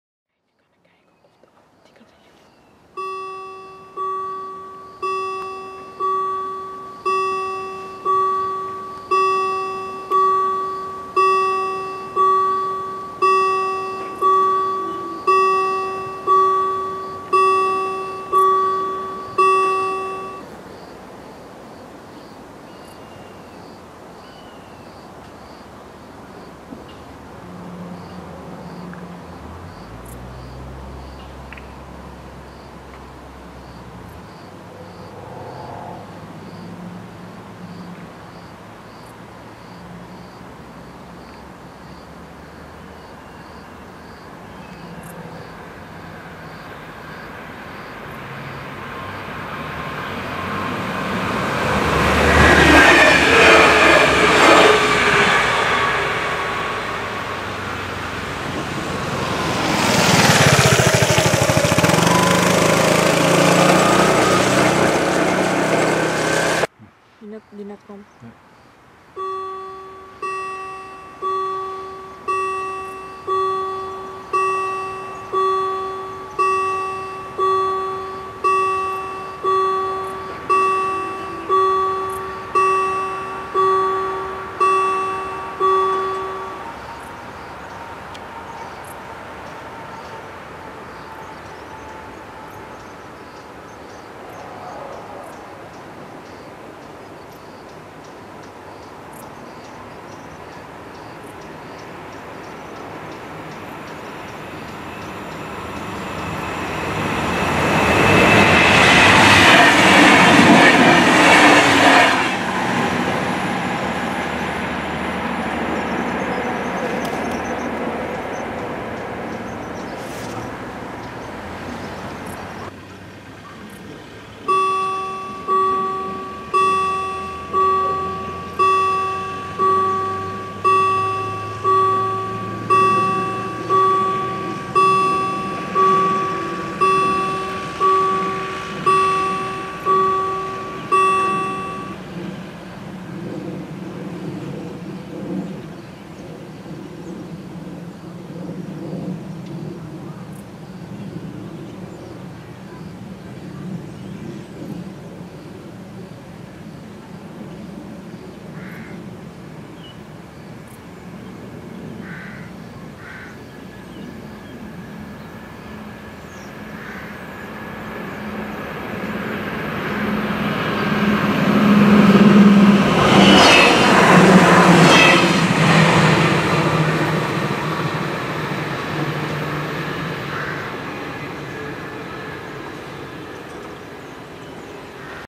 Spoorwegovergang / Bahnübergang Herten (Baden, D)
Passage van DB treinen (respectievelijk BR641, BR612 en BR644) bij spoorwegovergang in Herten bij Rheinfelden in Duitsland